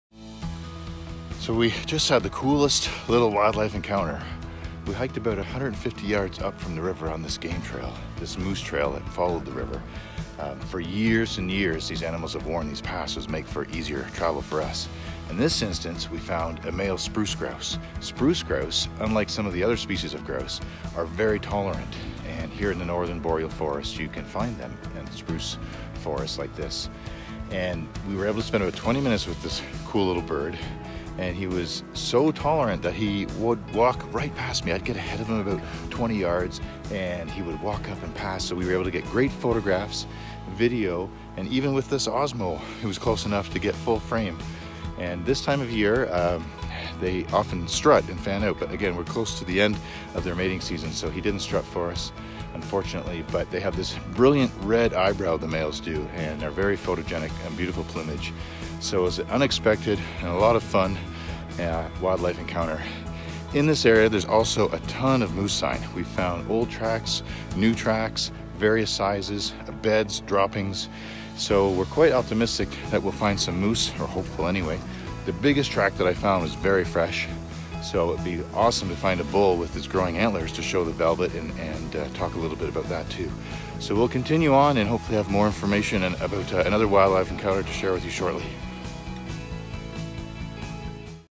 SURPRISED BY A SPRUCE GROUSE!
Mark and Ron find and film male spruce grouse in the old growth forest in the wild Northern Rockies!  Mark still hopes to find a moose based on all of the sign he’s seen on the hike.